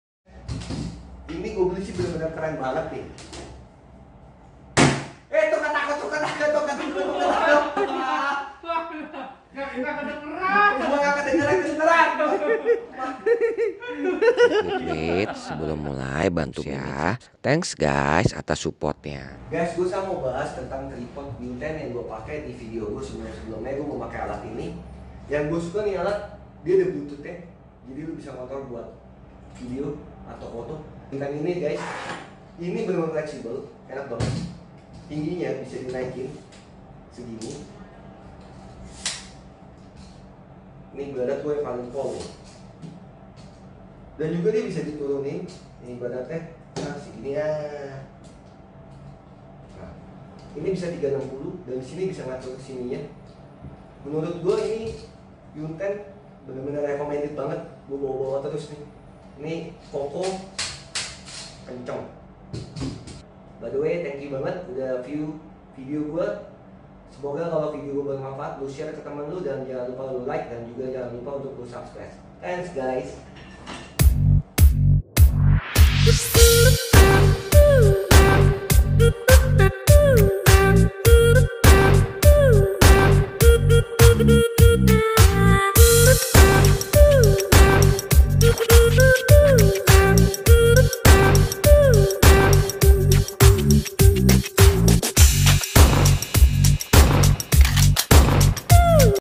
Review Tripod Yunteng Top Banget menurut Mimin
menurut mimin tripod merek Yunteng VC,
mimin suka karena dia ada bluetootnya dan kokoh banget bener2 TOP